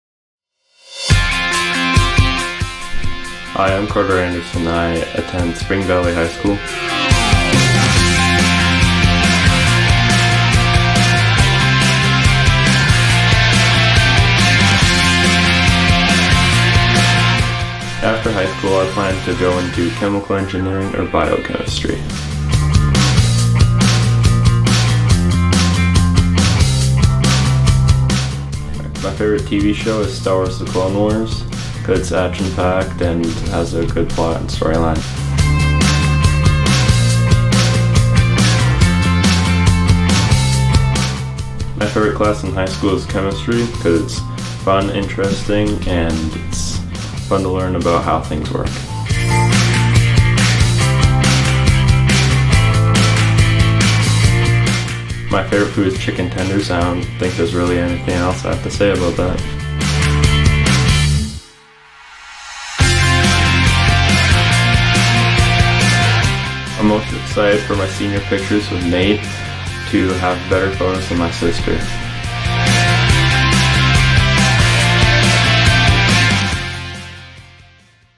Carter | Class of 2025
Carter's Class of 2025 Senior Experience Portrait Presentation by NP Design & Photography Tell Your Story!  1X Wisconsin and 3X Twin Cities Senior Photographer of the Year